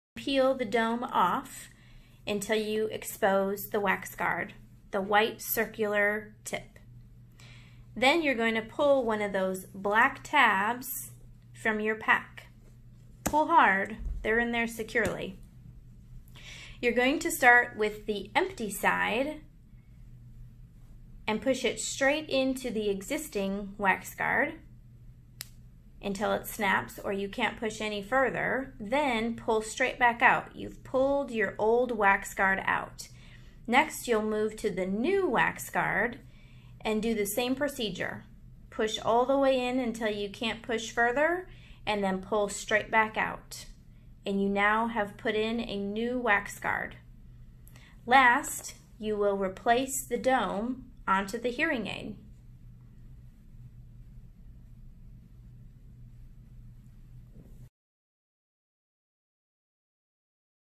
How to Change Oticon Wax Guards
If your hearing aid sounds weak or dead, changing the wax guard may solve the problem. Your audiologist can supply you with wax guards if you do not have any.